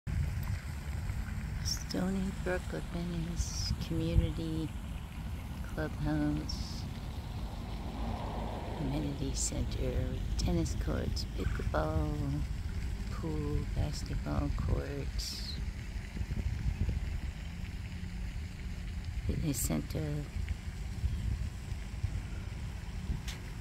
Stoneybrook at Venice Community
I have a new listing in this community

 5 bedrooms
 3.5 bathroom
2 car garage
Lake view

fixer upper
2930 sq ft